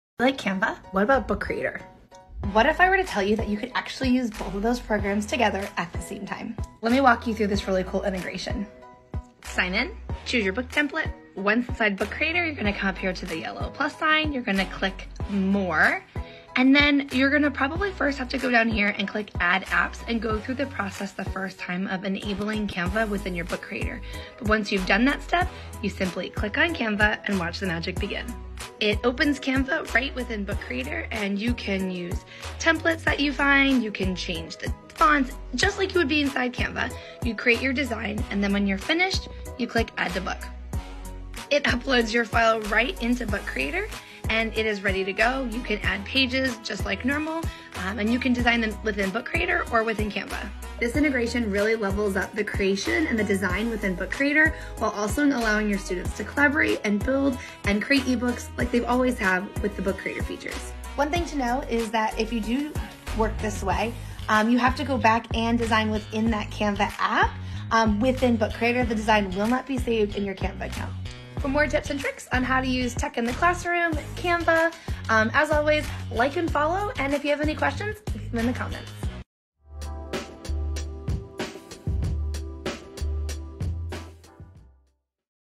How to Use Canva with Book Creator
@TheMerrillsEDU share how to use @canva inside @BookCreatorApp!